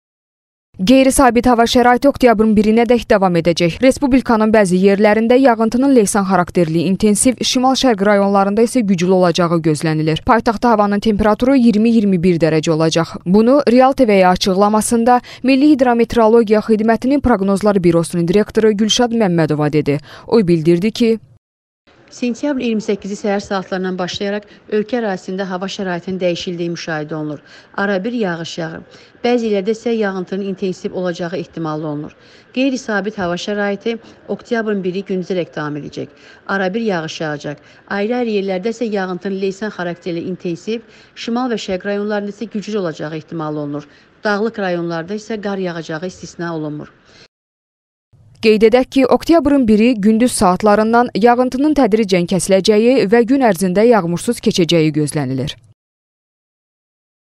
OKTYABRIN 1 DƏK YAĞIŞ YAĞACAQ
Bəzi yerlərdə intensiv, leysan xarakterli olacaq